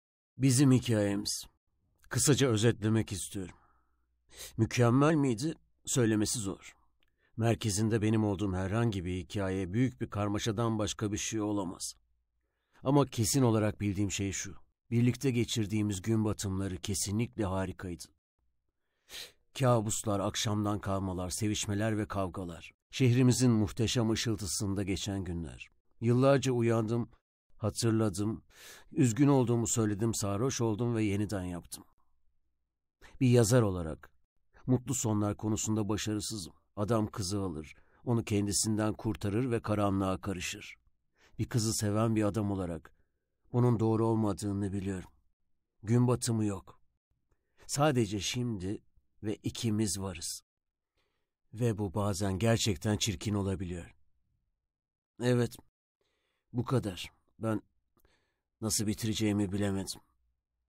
Californication
Uslanmaz çocuk Hank Moody’nin çılgın ve eğlenceli hikayesini 7 sezon konuştuk. Netflix’te yayında.
Hank: Kerem Kobanbay
Karen: Zeynep Ateşer
Charlie: Vural Buldu